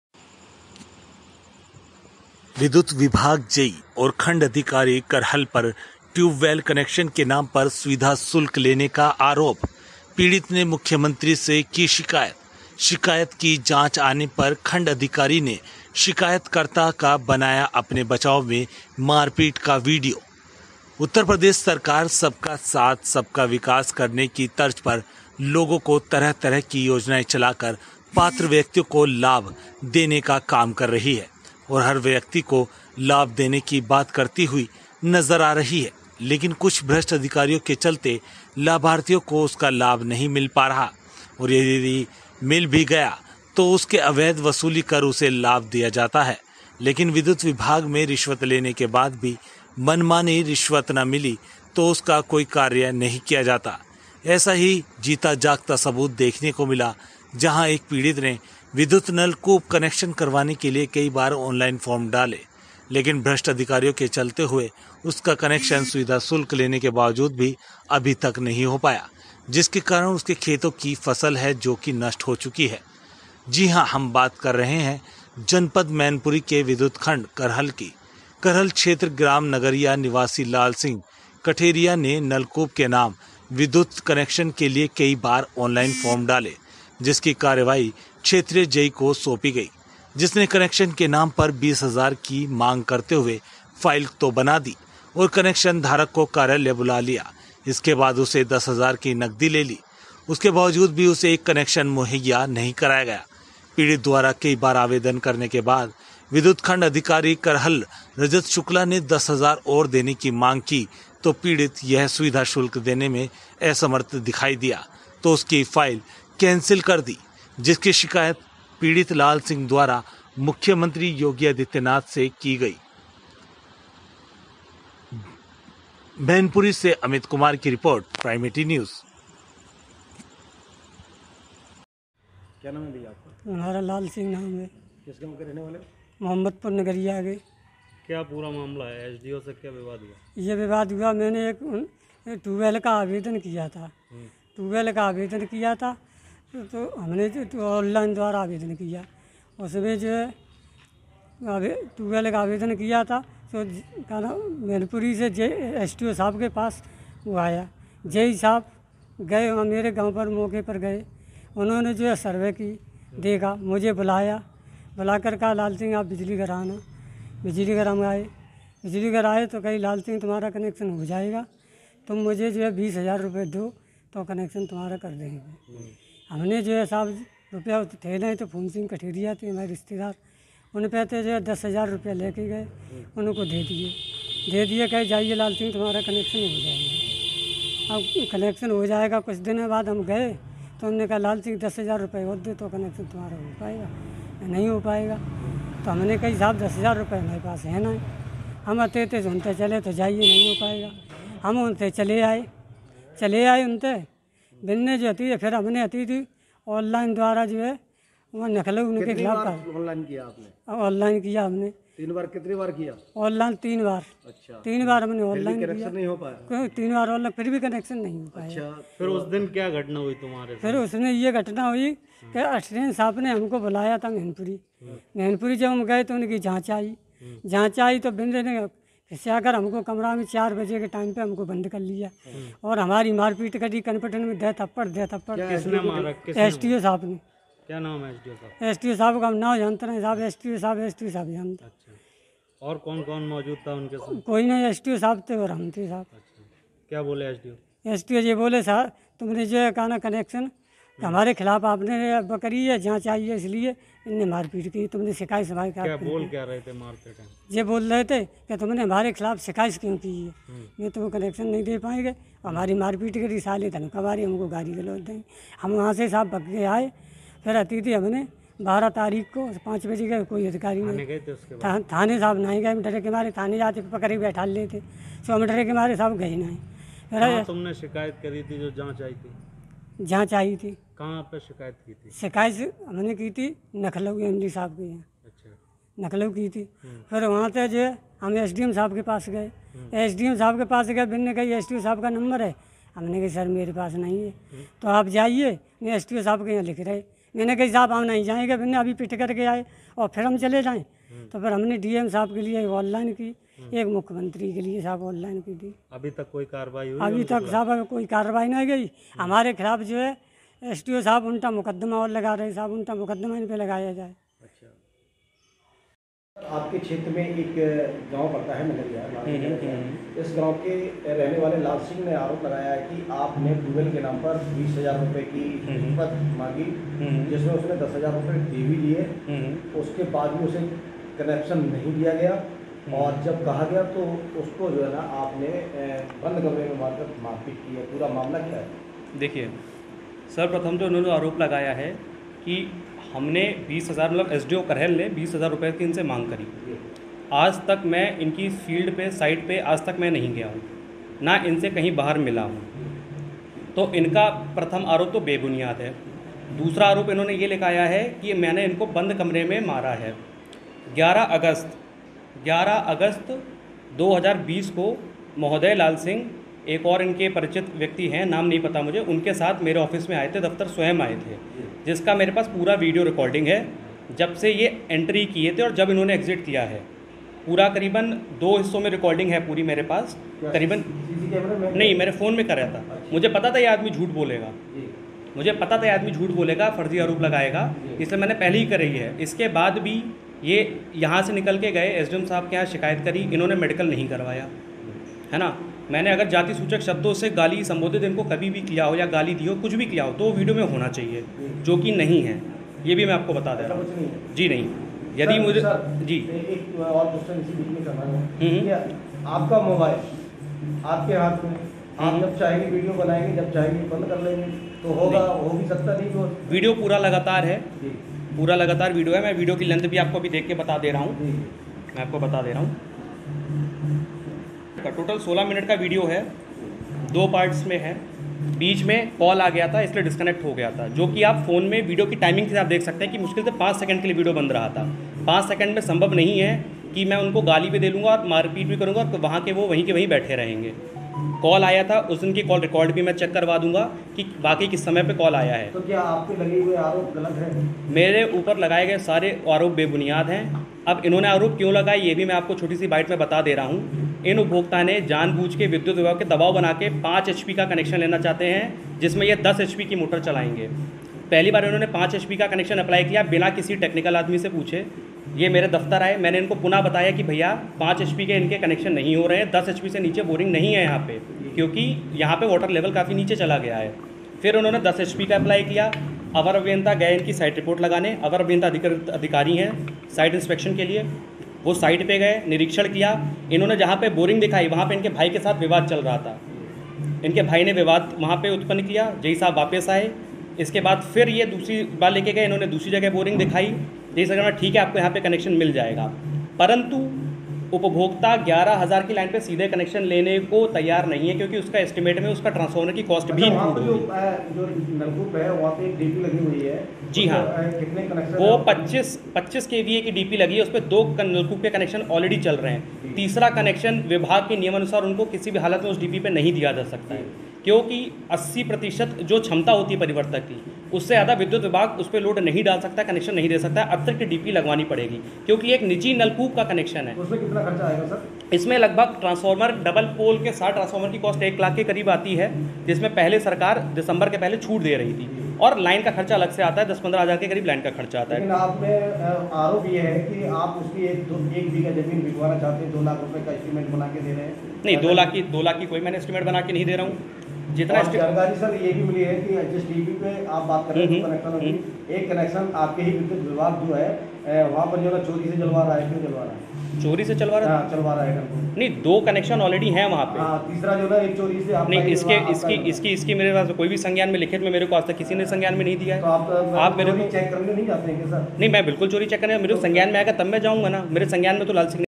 विद्युत विभाग जेई और खंड अधिकारी करहल पर ट्यूबवेल कनेक्शन के नाम पर सुविधा शुल्क लेने का आरोप
रिपोर्टर अमित कुमार
 जनपद मैनपुरी
दिनांक 3 सितंबर 2020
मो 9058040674
स्लोगन
पीड़ित ने मुख्यमंत्री से की शिकायत शिकायत की जांच आने पर खंड अधिकारी ने शिकायतकर्ता का बनाया अपने बचाव में मारपीट कर वीडियो
एंकर
उत्तर प्रदेश सरकार सबका साथ सबका विकास करने की तर्ज पर लोगों को तरह-तरह की योजनाएं चलाकर पात्र व्यक्तियों को लाभ देने का काम कर रही है और हर व्यक्ति को  लाभ देने की बात करती हुई नजर आ रही है लेकिन कुछ भ्रष्ट अधिकारियों के चलते लाभार्थियों को उसका लाभ नहीं मिल पा रहा है और यदि मिल भी गया तो उससे अवैध वसूली कर उसे लाभ दिया जाता है लेकिन विद्युत विभाग में रिश्वत लेने के बाद भी  मनमानी रिश्वत ना मिली तो  उसका कोई कार्य नहीं किया जाता है  ऐसा ही गीता जाता  सबूत देखने को मिला जहां एक पीड़ित ने विद्युत नलकूप कनेक्शन करवाने के लिए कई बार ऑनलाइन फॉर्म डाले लेकिन भ्रष्ट अधिकारियों के चलते हुए उसका कनेक्शन सुविधा शुल्क लेने के बावजूद भी अभी तक नहीं हो पाया है जिसके कारण उसके खेतों की फसल है नष्ट हो चुकी है
 जी हां हम बात कर रहे हैं जनपद मैनपुरी के विद्युत खंड करहल की करहल क्षेत्र ग्राम नगरिया निवासी लाल सिंह कठेरिया ने नलकूप के नाम विद्युत कनेक्शन के लिए कई बार ऑनलाइन फॉर्म डाले जिसकी कार्रवाई क्षेत्रीय जेई को सौंपी गई जिसने कनेक्शन के नाम पर 20,000 की मांग करते हुए फाइल तो बना दी  और कनेक्शन धारक को कार्यालय बुला लिया इसके बाद उसे 10000 की नगदी ले ली इसके बावजूद भी उसे एक कनेक्शन मुहैया नहीं कराया गया पीड़ित द्वारा कई बार निवेदन करने के बाद विद्युत खंड अधिकारी करहल रजत शुक्ला ने 10,000 और देने की मांग की तो पीड़ित यह सुविधा शुल्क देने में असमर्थ दिखाई दिया तो उसकी फाइल कैंसिल कर दी जिसकी शिकायत पीड़ित लाल सिंह द्वार  मुख्यमंत्री योगी आदित्यनाथ से की गई
जिसकी जांच आने से पहले जेई और एसडीओ को भनक लग गई तो उसने पीड़ित को कार्यालय में बुलाकर अकेले कमरे में बंद कर लिया और मारपीट करते हुए उससे मनमाने तरीके से बुलवाकर एक उसका वीडियो बनाया जिससे वह निर्दोष साबित हो सके और पीड़ित के ही खिलाफ मुकदमा दर्ज करने की धमकी दे डाली इसकी सोच रहा जब मीडिया कर्मियों को लगी तो मौके पर पहुंची मीडिया ने दबंग विद्युत खंड अधिकारी रजत शुक्ला से बात की तो वह अपनी सफाई देते हुए किस तरह से बनाई गई वीडियो का जिक्र करते हुए नजर आ रहे हैं जरा गौर से सुनिए
इससे साफ जाहिर होता हुआ नजर आ रहा है कि इस तरह के भ्रष्ट अधिकारी अगर जनपद में रहेंगे तो पीड़ितों को सरकार द्वारा चलाई गई योजनाओं का लाभ कैसे मिल सकेगा नलकूप विद्युत के लिए आज भी पीड़ित  दर-दर की  ठोकरें खाता हुआ नजर आ रहा है देखें यह रिपोर्ट
वाइट- पीड़ित लाल सिंह कठेरिया
बाइट -फूल सिंह कठेरिया
वाइट -रजत शुक्ला विद्युत खंड अधिकारी करहल